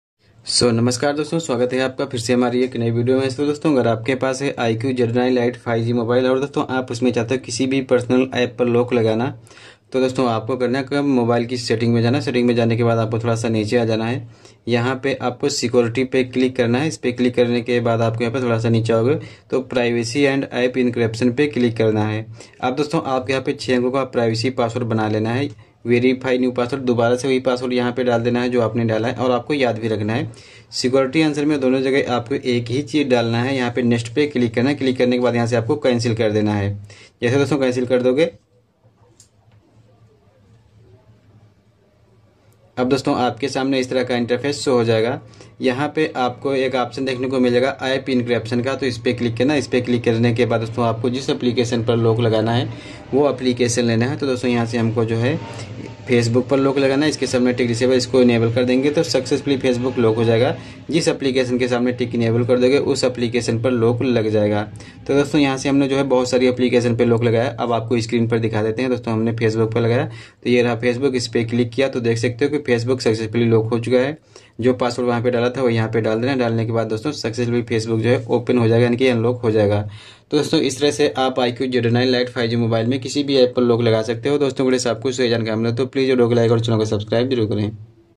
iqoo Z9 Lite 5g me app lock kaise lagaye/how to set app lock iqoo Z9 Lite 5g
hello dosto aaj ki is video ke madhyam se Maine aap logo ko bataya hai ki iqoo Z9 Lite 5g mobile me app lock kaise lagaya jata hai